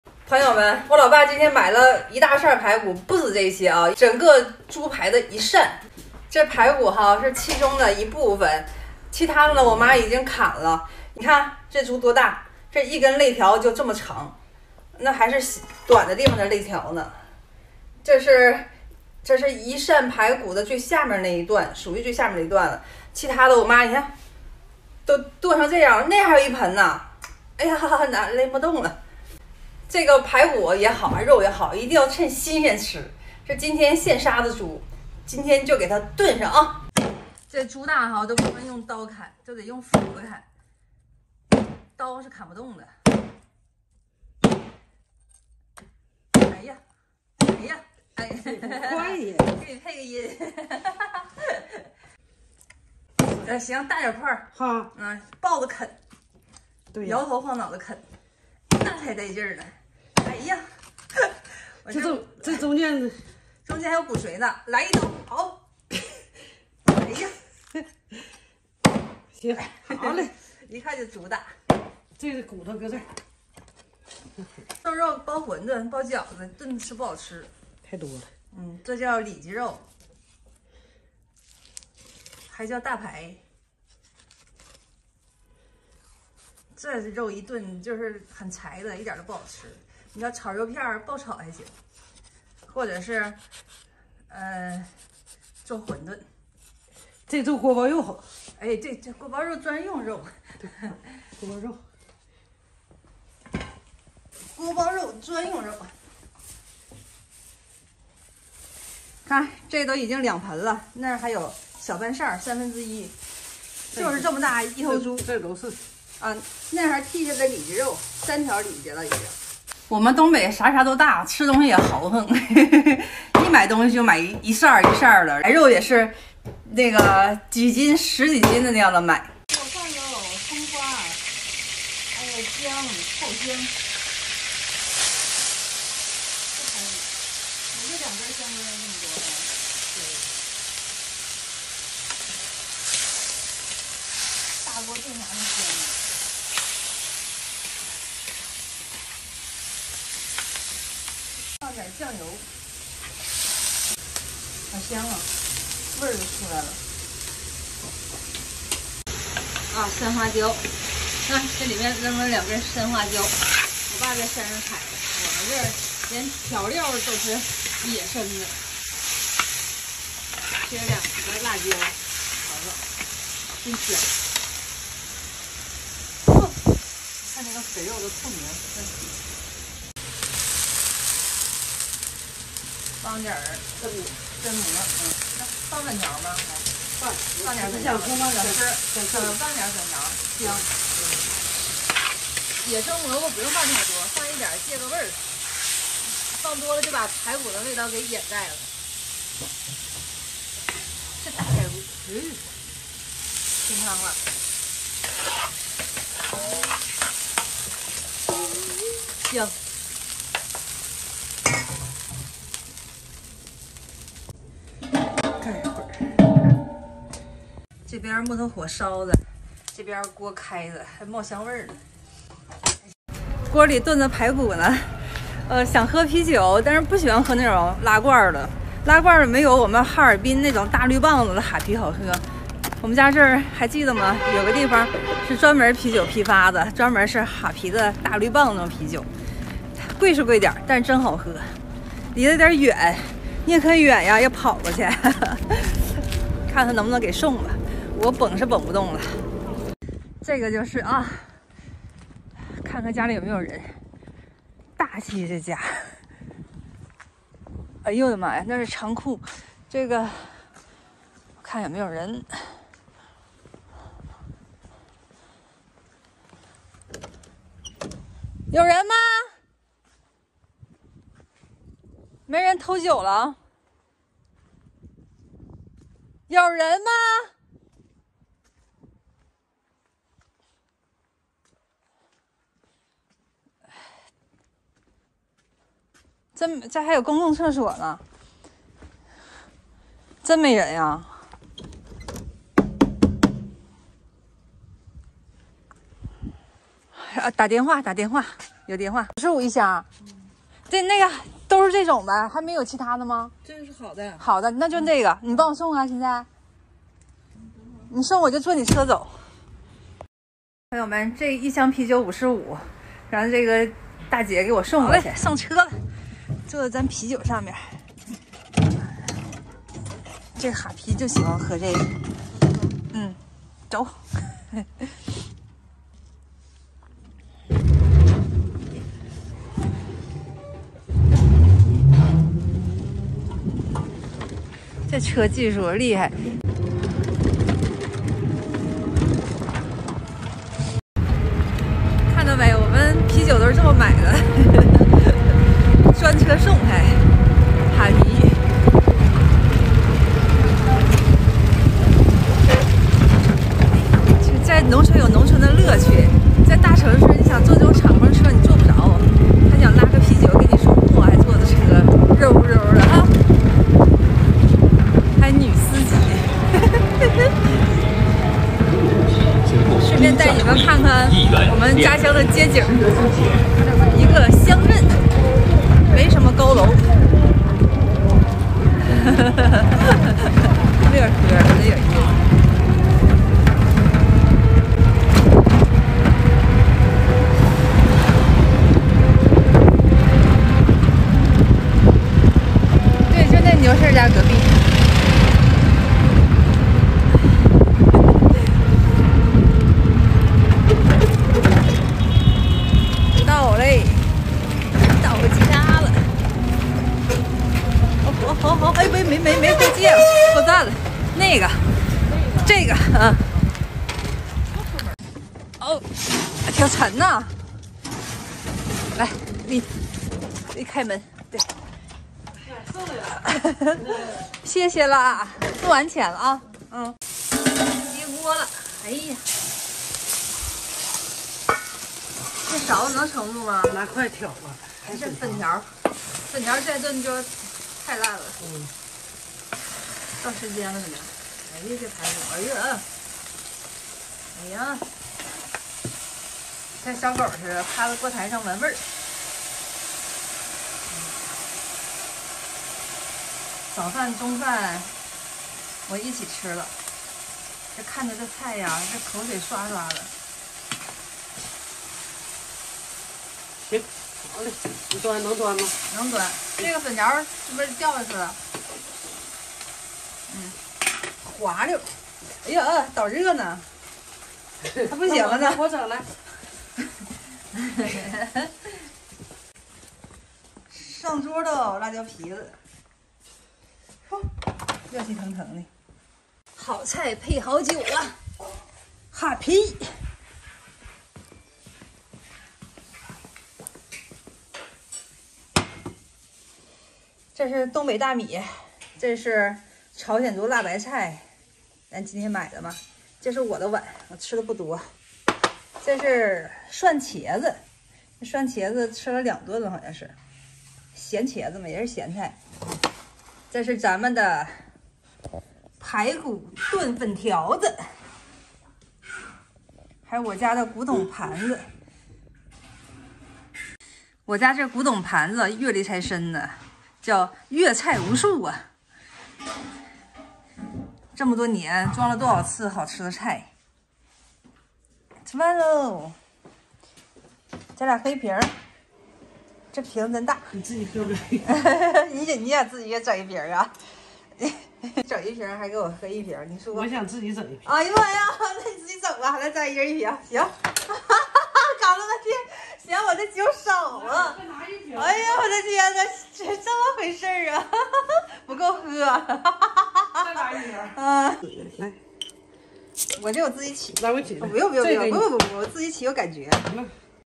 400多斤的大猪，花200元买了一扇排骨，老妈做了一锅排骨炖粉条【莹莹】
400多斤的大猪，花200元买了一扇排骨，老妈做了一锅排骨炖粉条【莹莹】
莹莹每天与大家分享生活旅游见闻和美食搜罗视频